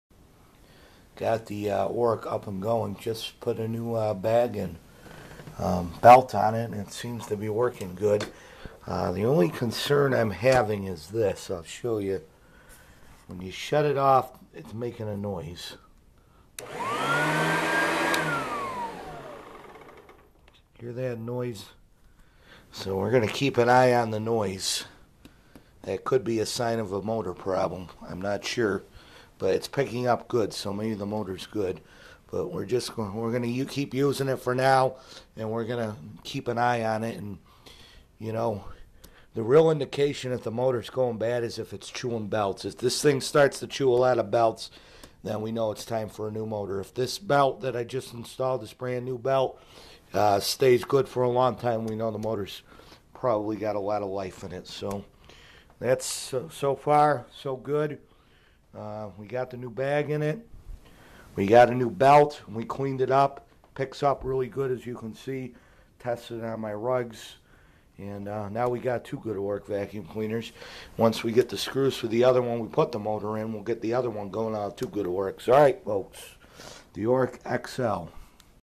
Got new belt and bag installed on xl Oreck
Keep a I on it if it chews belt then needs motor if belt last while motor is most likely good good suction.